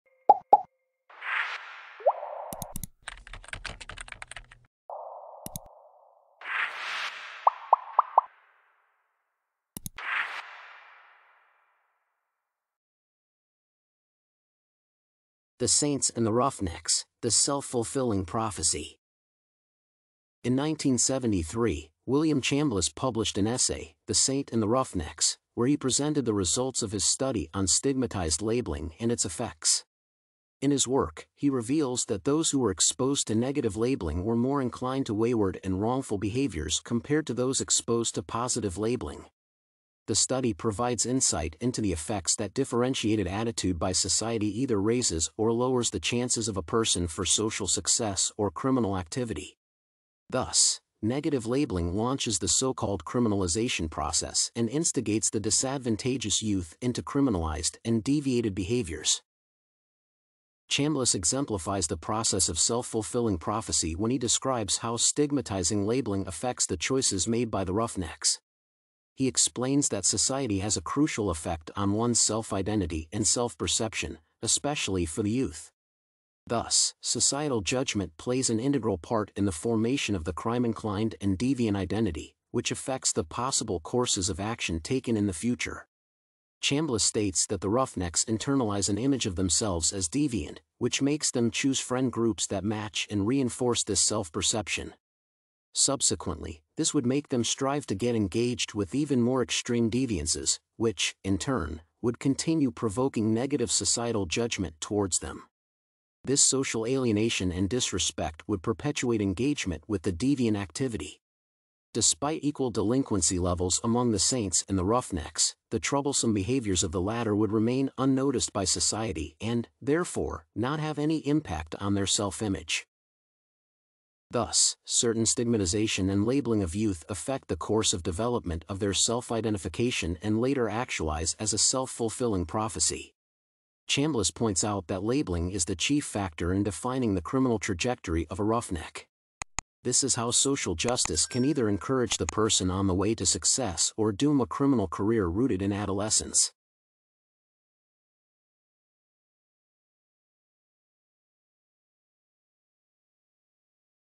“The Saints and the Roughnecks”: The Self-Fulfilling Prophecy - Essay Example
📝 Essay description: “The Saints and the Roughnecks” states that labeling of youth affects the course of development of their self-identification and later actualizes it as a self-fulfilling prophecy.

ℹ️ Subjects: Organizational Behavior
ℹ️ Words: 335
ℹ️ Pages: 1

📚 Reference list:
Chambliss, W. (1973). The Saints and the Roughnecks. Society 11, 24-31. Web.